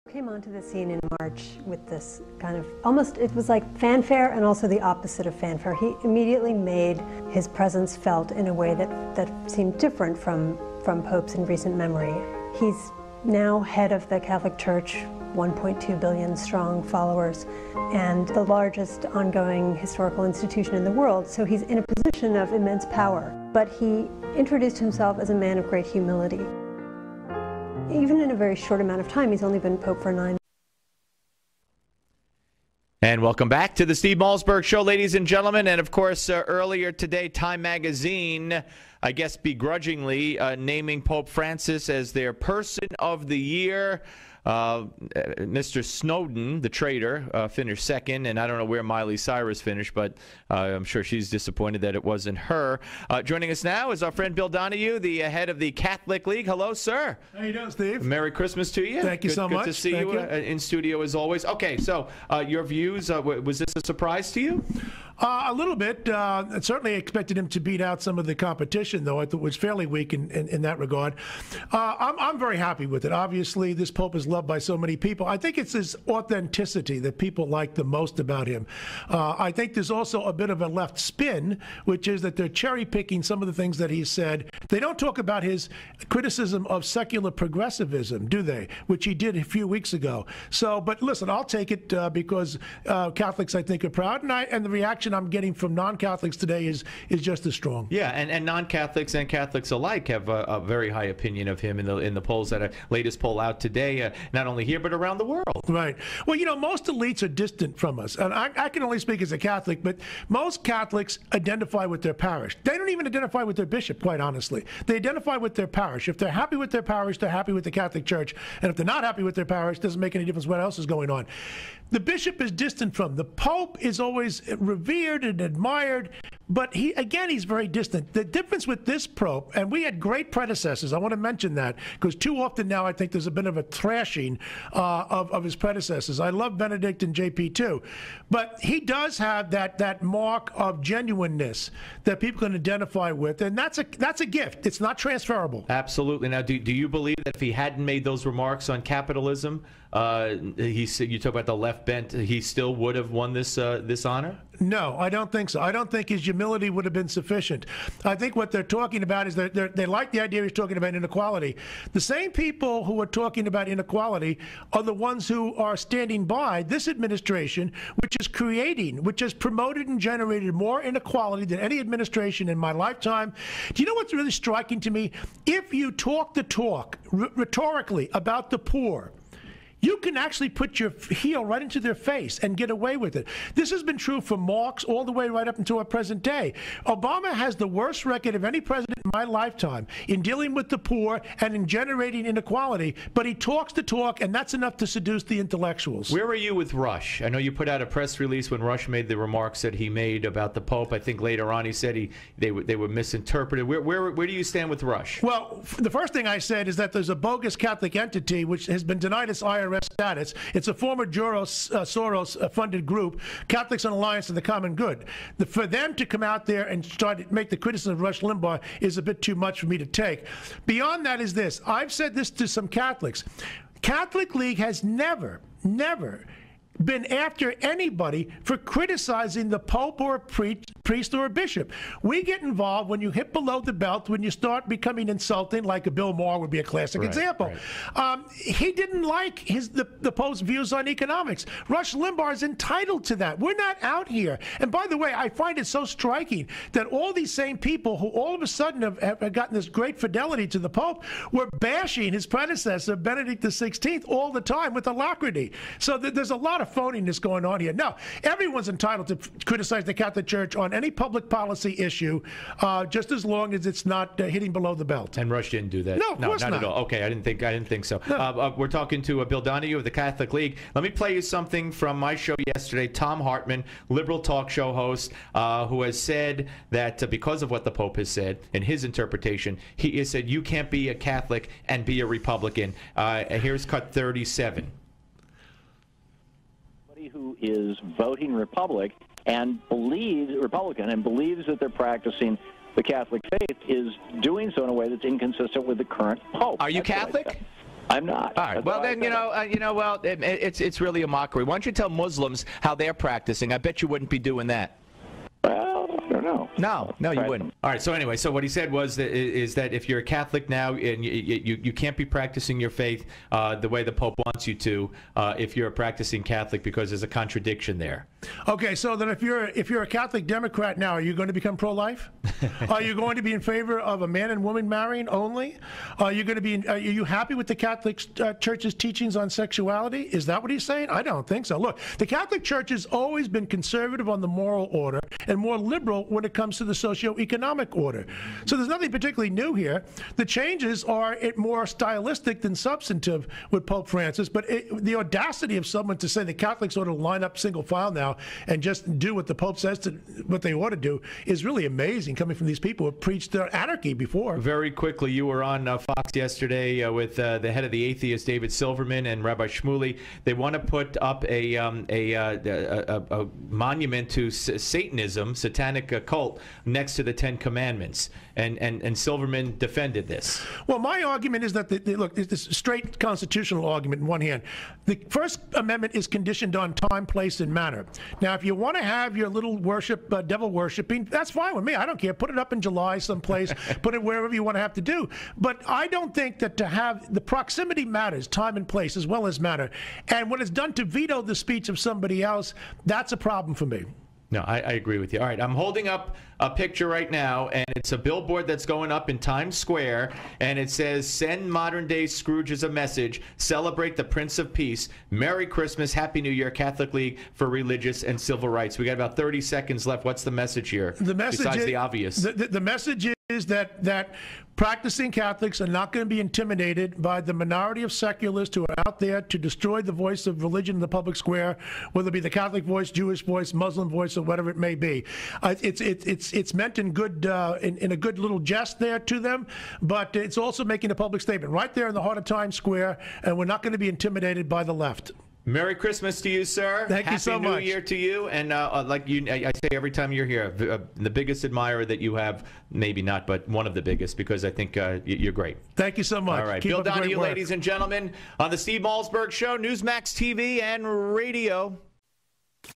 Bill Donohue - President of The Catholic League
joins Steve to discuss Pope Francis being named Time Magazine's "Person of the Year"!